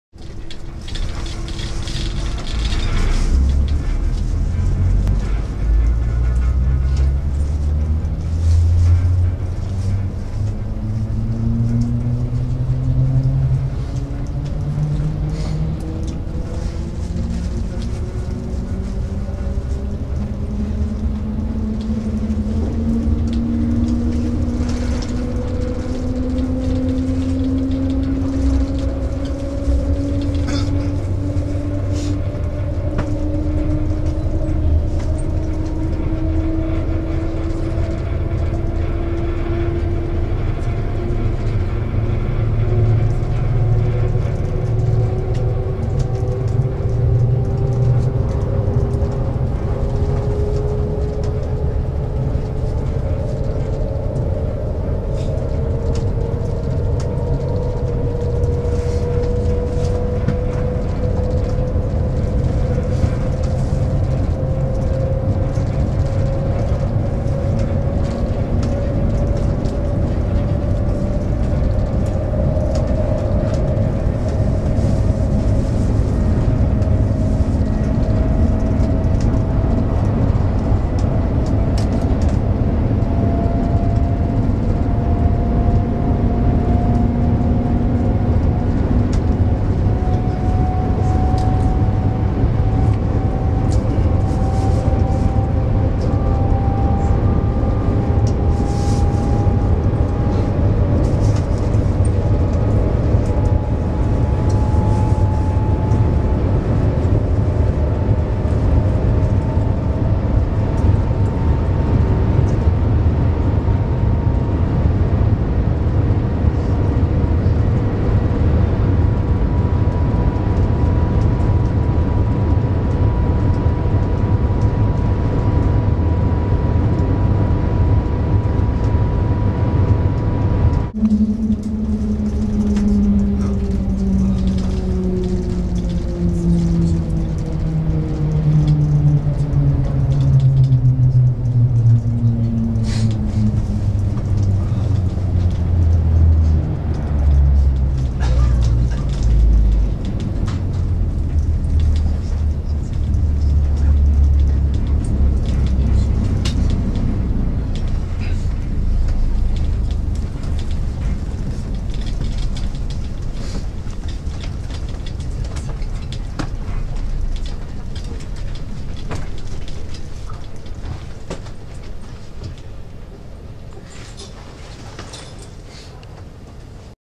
Shanghai's Maglev Train
Shanghai's Maglev (short for magnetic levitation) Train is convenient and quick, connecting Shanghai's Pudong International Airport with the outskirts of the city, a distance of 30.5kms, in 8 minutes with speeds of up to 430km/hour.